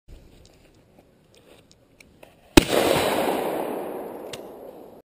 Trap, 3 stanowisko, Kamil z boku, szybko.
Pokaz szybkiego strzelania na Trapie